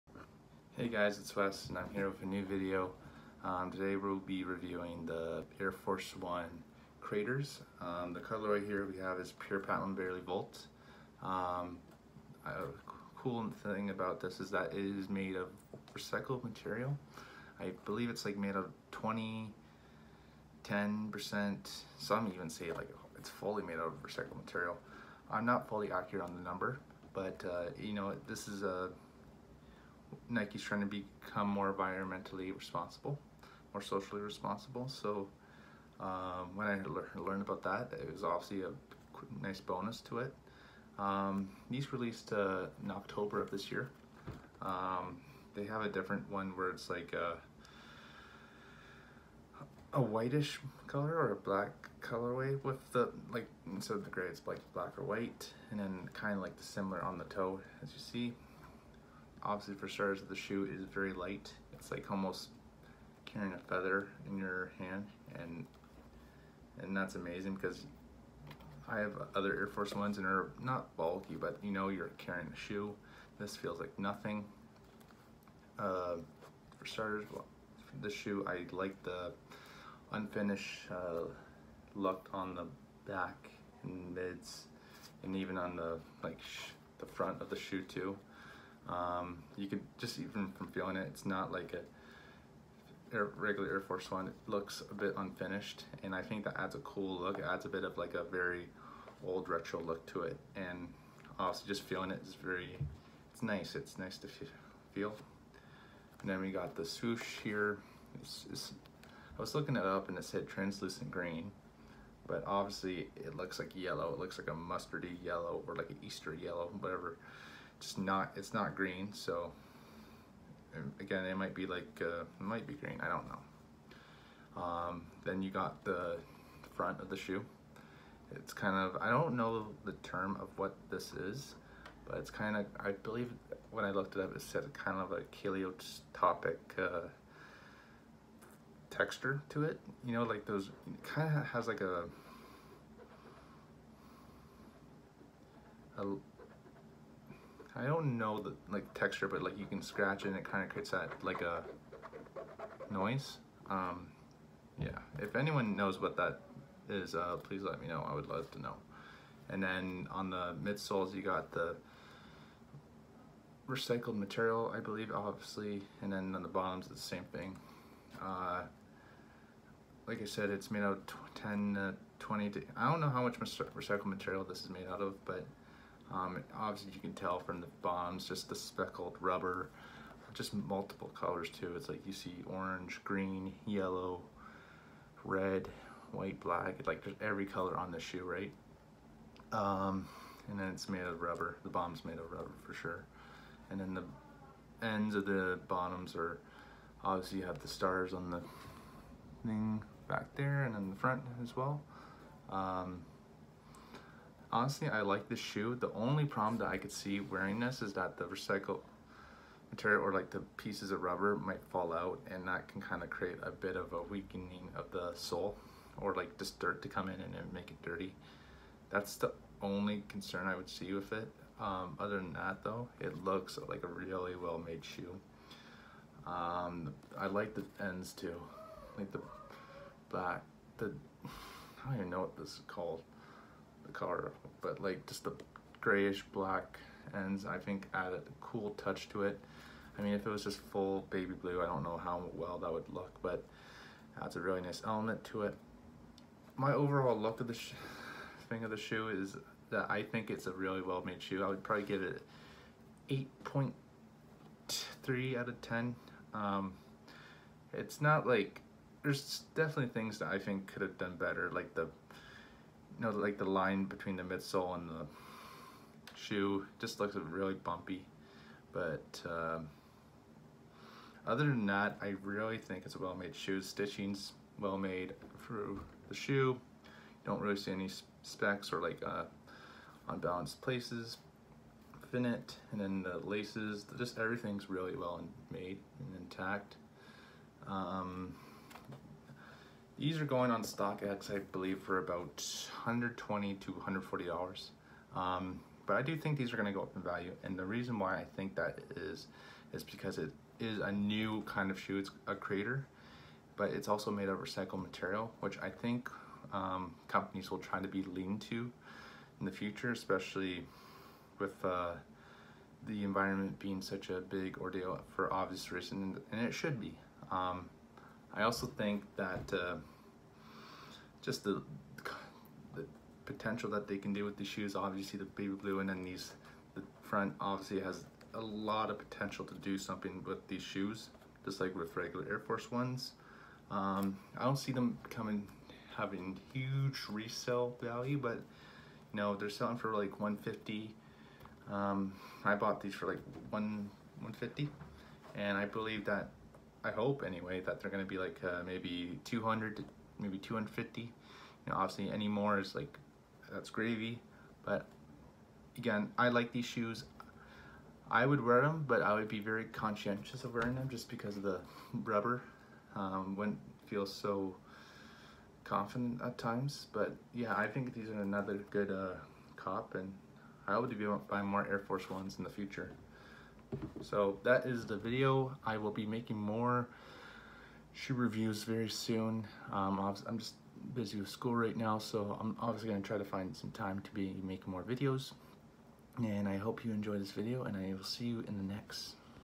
Nike AF1 Crater Review -Pure Platnuim / Barley Volt
Hello Wonderful people , This video I am reviewing the Nike Air Force 1 Crater , Pure Platinum / Barely Volt. I hope you enjoy this video and I plan to be making more videos when I can.
Sneaker Instagram:DripDropzs
Personal:Wesdobbin